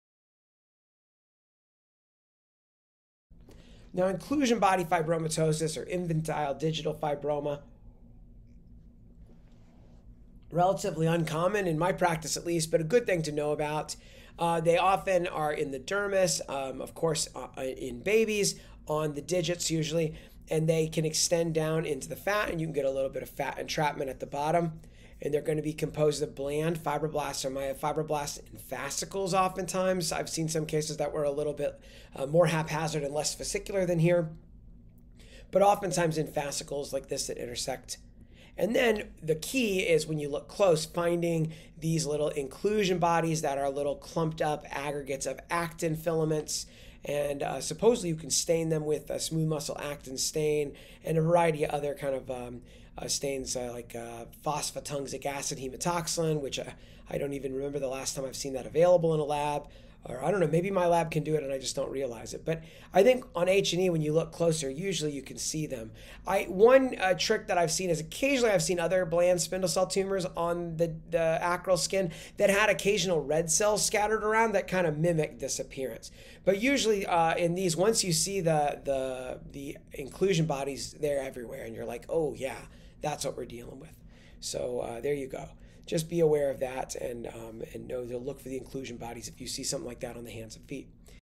Inclusion Body Fibromatosis (Infantile Digital Fibroma/tosis) in 5 Minutes (dermpath pathology)
This is an excerpt from my fibrohistiocytic dermpath 101 video (full 1.5 hour video presented virtually at the 2022 Australasian Dermatopathology Society Meeting.